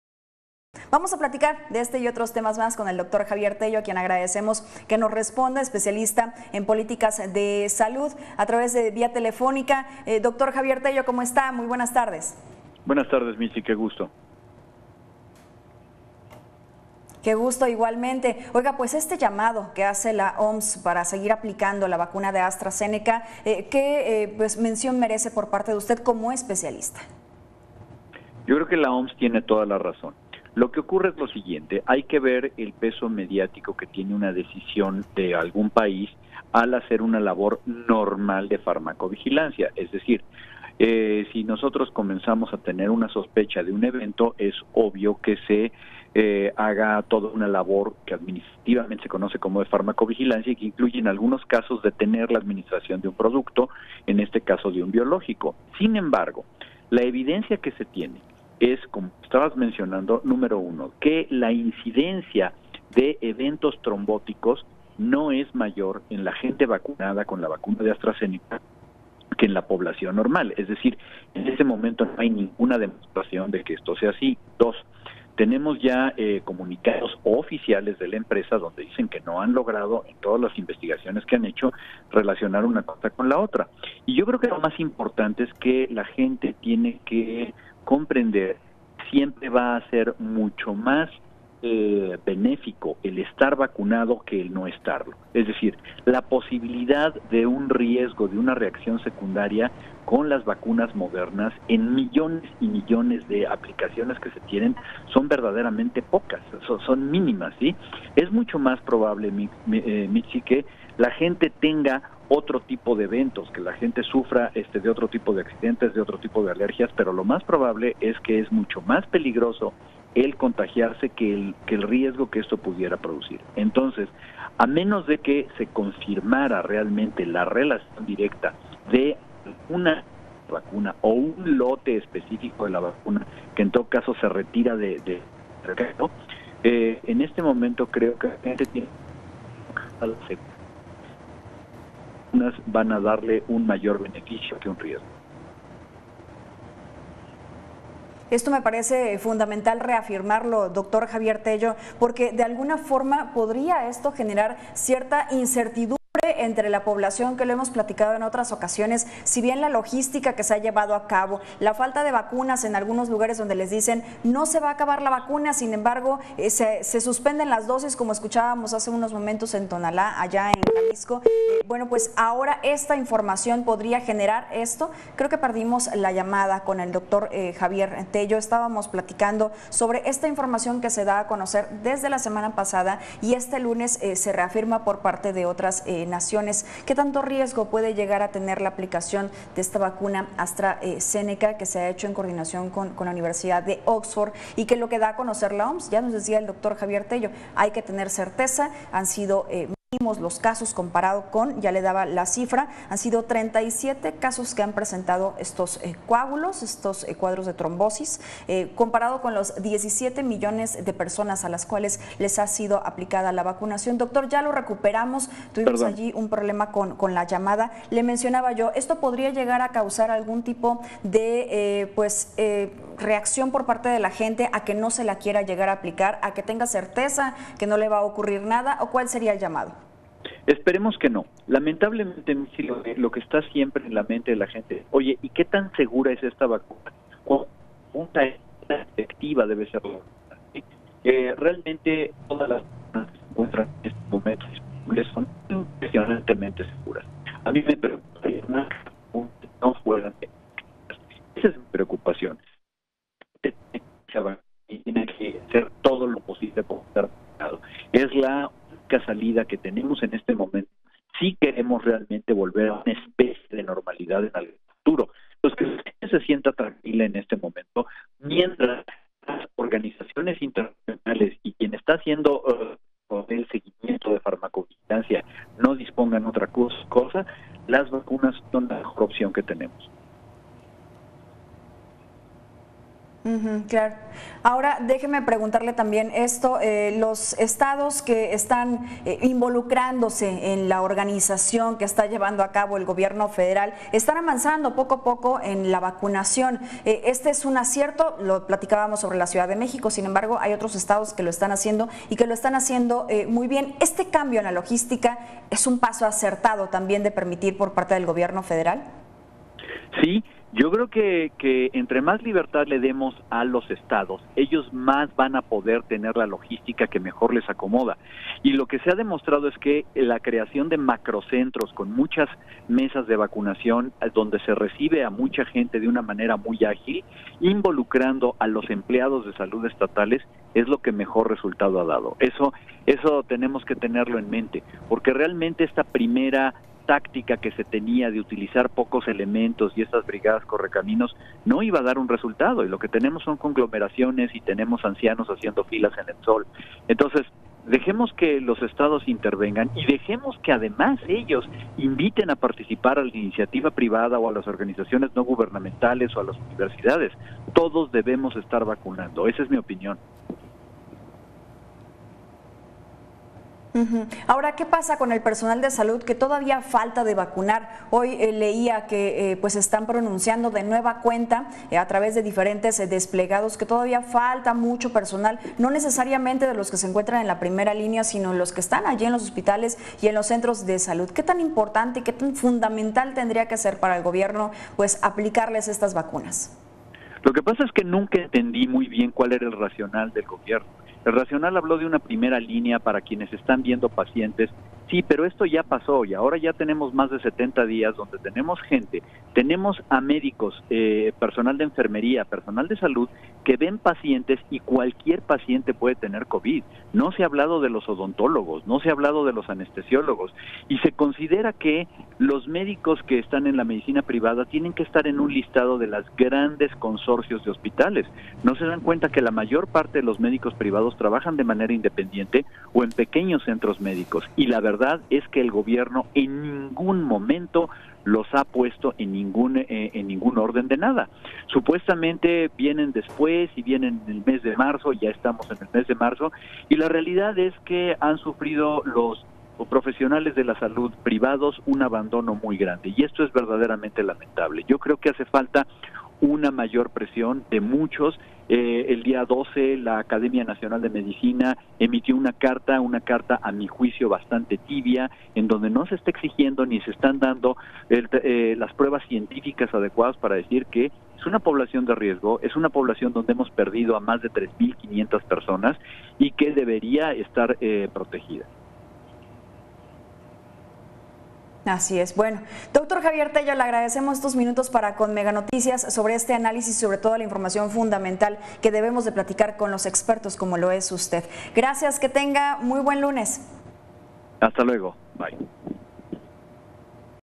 Suspensión preventiva de vacuna contra COVID por presencia de coágulos en algunas personas vacunadas
Mitzi Cordero en entrevista con Dr. Xavier Tello, Especialista en políticas de Salud